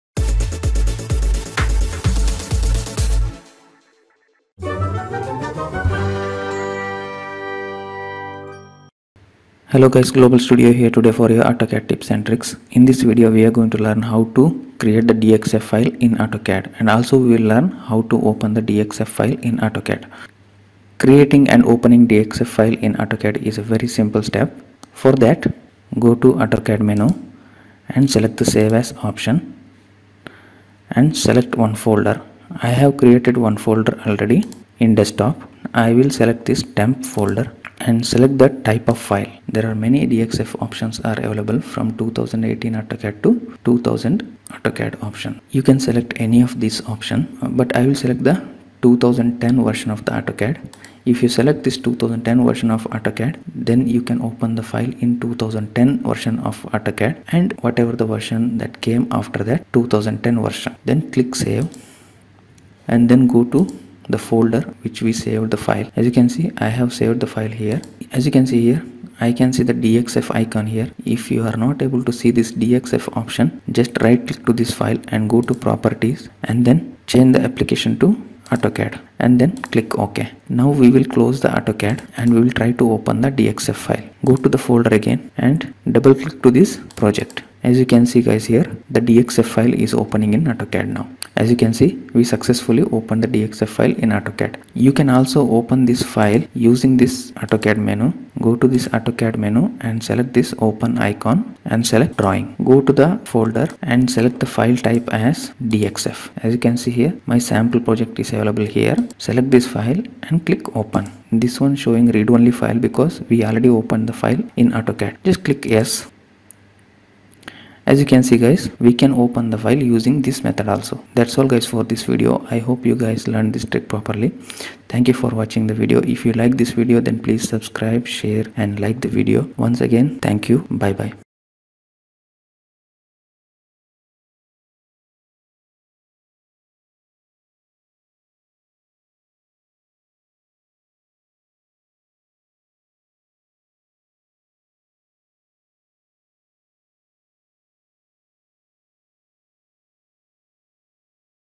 HOW TO EXPORT AND IMPORT DXF FILE IN AUTOCAD
In this video we will discuss how to export and import DXF files in autocad.